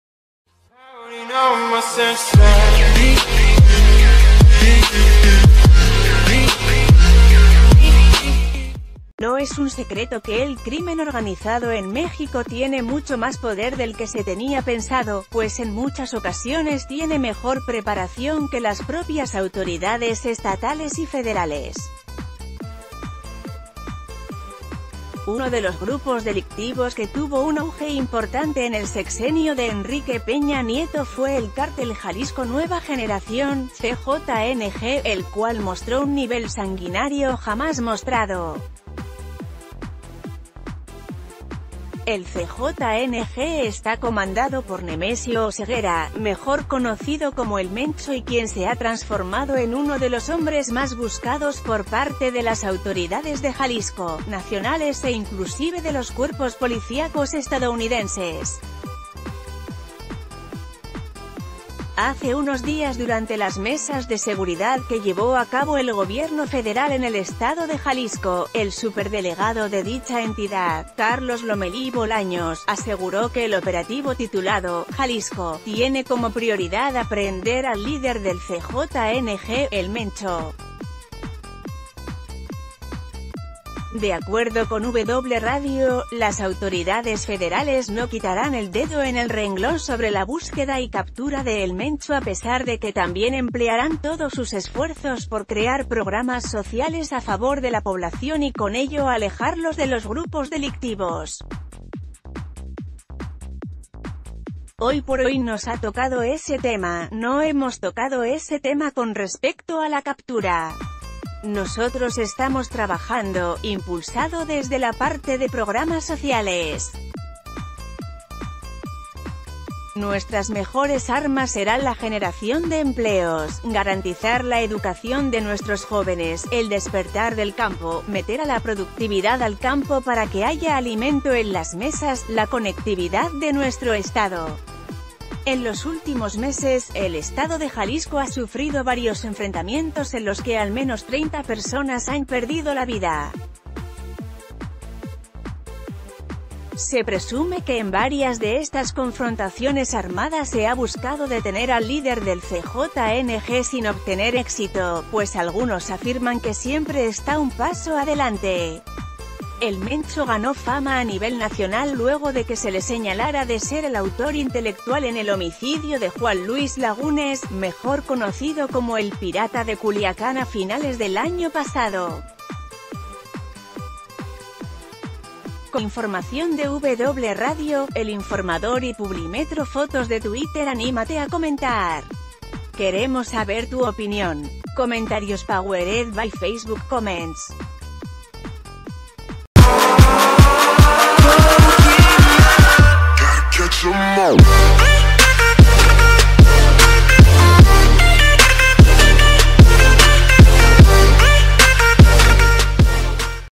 Detallan postura del Gobierno Federal respecto a la captura de El Mencho
No es un secreto que el crimen organizado en México tiene mucho más poder del que se tenía pensado, pues en muchas ocasiones tiene mejor preparación que las propias autoridades estatales y federales. Uno de los grupos delictivos que tuvo un auge importante en el sexenio de Enrique Peña Nieto fue el ...